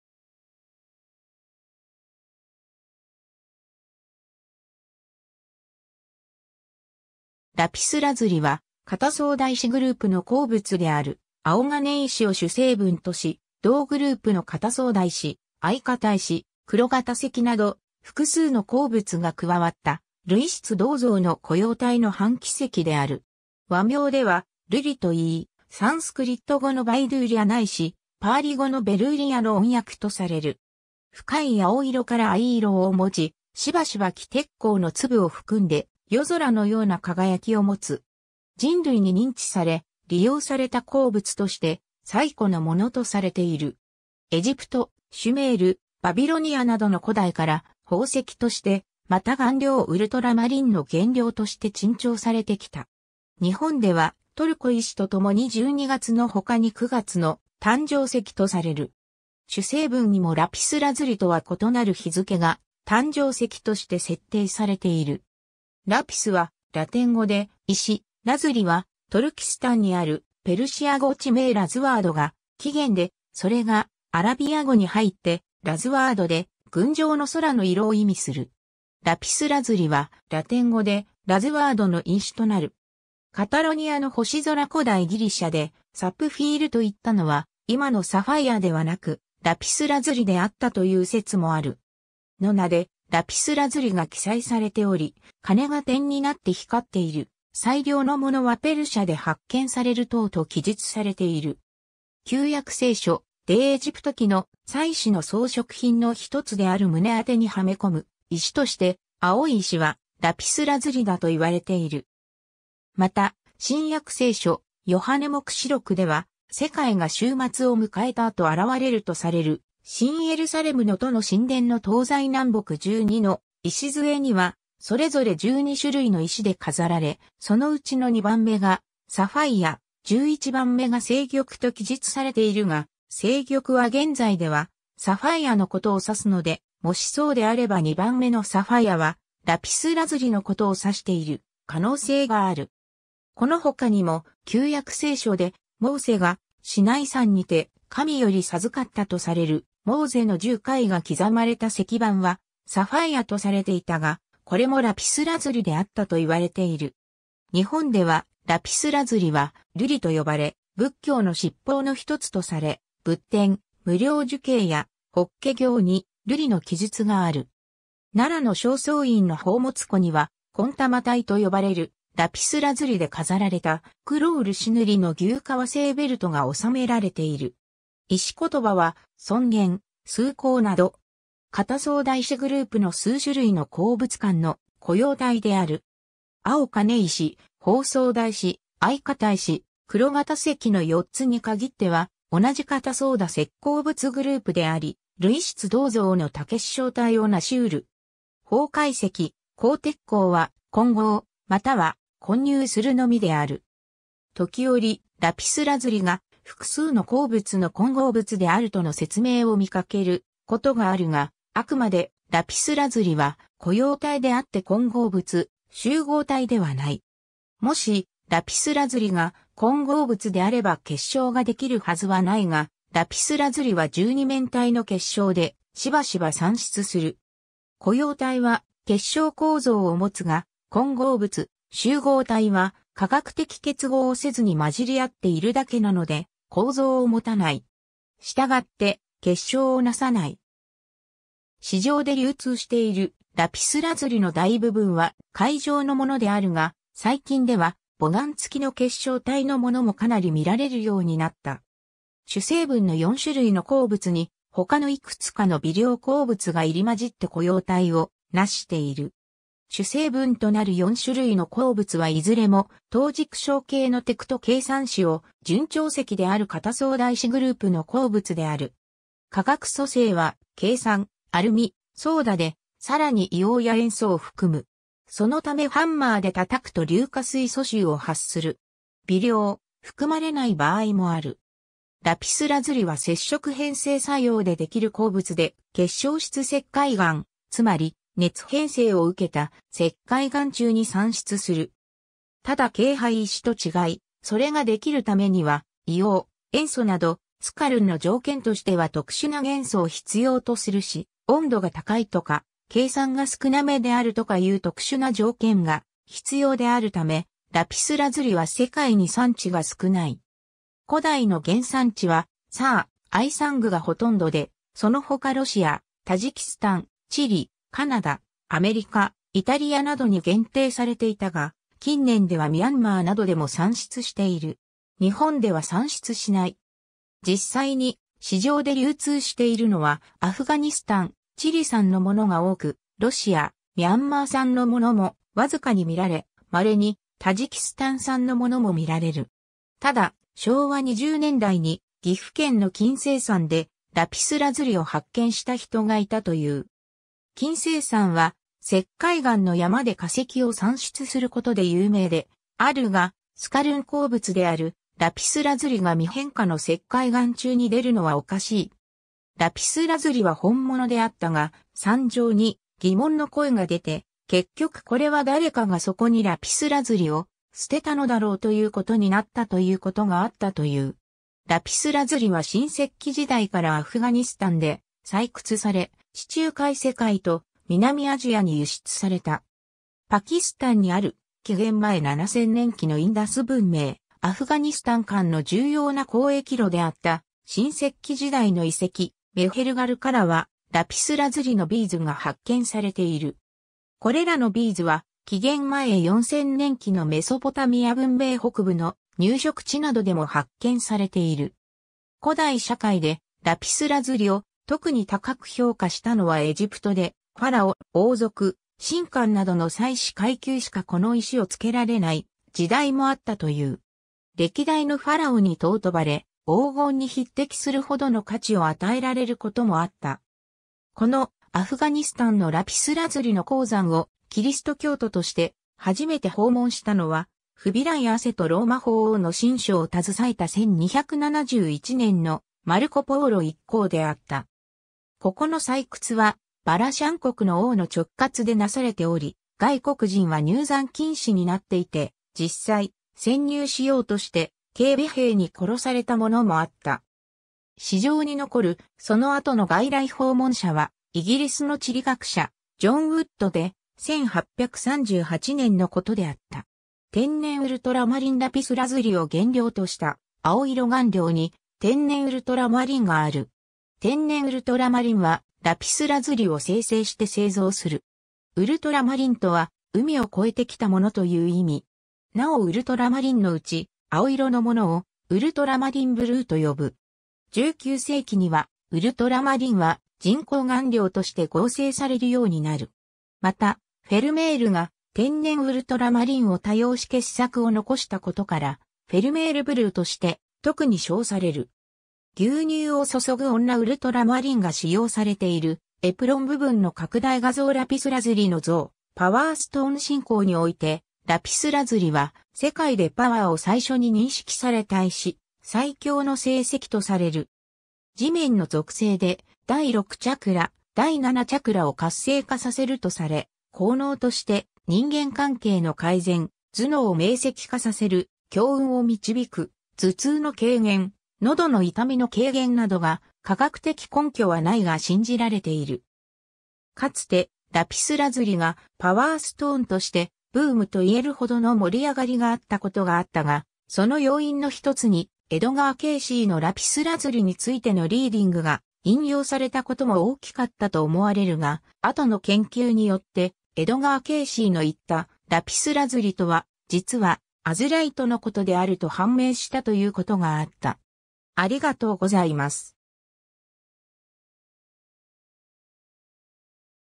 ラピスラズリ
ラピスラズリ (lapis lazuli) は、方ソーダ石グループの鉱物である青金石（ラズライト）を主成分とし、同グループの方ソーダ石・藍方石・ 黝方石（ゆうほうせき）など複数の鉱物が加わった類質同像の固溶体の半貴石である。
和名では 瑠璃（るり）といい、サンスクリット語のヴァイドゥーリャないしパーリ語のヴェルーリヤの音訳とされる。
深い青色から藍色を持ち、しばしば黄鉄鉱の粒を含んで夜空のような輝きを持つ。
 人類に認知され、利用された鉱物として最古のものとされている。
エジプト、シュメール、バビロニアなどの古代から、宝石として、また顔料ウルトラマリンの原料として珍重されてきた。
日本ではトルコ石と共に12月のほかに9月の誕生石とされる。
主成分にもラピスラズリとは異なる日付が誕生石として設定されている。
 ラピスはラテン語で「石」 (Lapis)、ラズリはトルキスタンにあるペルシア語地名 "lazhward"（ペルシア語：لاژورد‎、現在のアフガニスタン・イスラム共和国バダフシャーン州en:Kuran wa Munjan DistrictにあるSar-i Sang鉱山の古名）が起源で、それがアラビア語に入って "lazward"（アラビア語：لازورد‎、ラズワルド: 天・空・青などの意でアジュールの語源）で「群青の空の色」を意味する。
ラピスラズリ (lapis lazuli) は、ラテン語で「lazhwardの石 (lapis)」となる。
 カタロニアの星空 古代ギリシャでサプフィールといったのは、今のサファイアではなく、ラピスラズリであったという説もある。
（古代ローマの大プリニウスが著した博物誌には、サッフィール（サッピルス）の名でラピスラズリが記載されており、「金が点になって光っている」、「最良のものはペルシャで発見される」等と記述されている。
） 旧約聖書『出エジプト記』の、祭司の装飾品のひとつである胸当てにはめ込む石として青い石（sappir）は、ラピスラズリだといわれている。
また新約聖書『ヨハネ黙示録』では、世界が終末を迎えた後現れるとされる新エルサレムの都の神殿の東西南北12の礎にはそれぞれ12種類の石で飾られ、そのうちの2番目がサファイア、11番目が青玉と記述されているが、青玉は現在ではサファイアのことを指すので、もしそうであれば2番目のサファイアはラピスラズリのことを指している可能性がある。
この他にも旧約聖書でモーセがシナイ山にて、神より授かったとされるモーゼの十戒が刻まれた石版はサファイアとされていたが、これもラピスラズリであったといわれている。
 日本では、ラピスラズリは瑠璃と呼ばれ、仏教の七宝のひとつとされ、仏典『無量寿経』や『法華経』に瑠璃の記述がある。
 奈良の正倉院の宝物庫には、紺玉帯と呼ばれるラピスラズリで飾られた黒漆塗の牛革製ベルトが収められている。
 石言葉は「尊厳・崇高」など。
 方ソーダ石グループの数種類の鉱物間の固溶体である。
青金石・方ソーダ石・藍方石・黝方石の4つに限っては、同じ方ソーダ石鉱物グループであり、類質同像の多結晶体をなしうる。
方解石、黄鉄鉱は「混合」または「混入」するのみである。
 時折ラピスラズリが、複数の鉱物の混合物（岩石）であるとの説明を見かけることがあるが、あくまでラピスラズリは固溶体（solid solution）であって混合物／集合体（mixture）ではない。
もしラピスラズリが混合物（岩石）であれば結晶が出来るはずはないが、ラピスラズリは十二面体の結晶でしばしば産出する。
 固溶体（solid solution）は結晶構造を持つが、混合物／集合体（mixture）は化学的結合をせずに混じりあっているだけなので構造を持たない。
従って結晶をなさない。
 市場で流通しているラピスラズリの大部分は塊状のものであるが、最近では母岩付きの結晶体のものもかなり見られるようになった。
 主成分の4種類の鉱物に他のいくつかの微量鉱物が入り混じって固溶体をなしている。
主成分となる4種類の鉱物はいずれも等軸晶系のテクト珪酸塩・準長石である方ソーダ石グループの鉱物である。
化学組成は珪酸、アルミ、ソーダで、さらに硫黄や塩素を含む。
そのためハンマーで叩くと硫化水素臭を発する。
 微量、含まれない場合もある。
 ラピスラズリは接触変成作用でできる鉱物で、結晶質石灰岩、つまり熱変成を受けた石灰岩（スカルン）中に産出する。
ただ珪灰石と違い、それができるためには、硫黄、塩素など、スカ...